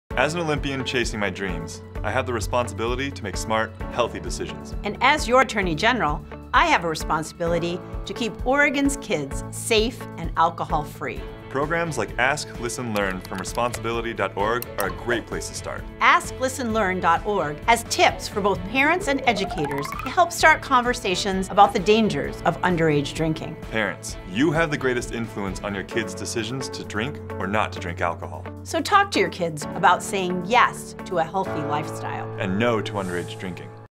Oregon Attorney General Ellen Rosenblum Joins Nathan Adrian to Prevent Underage Drinking
Nathan Adrian and Oregon Attorney General Elle Rosenblum want to encourage parents to talk with their kids about saying YES to a healthy lifestyle and NO to underage drinking.

Visit AskListenLearn.org to get the conversation started.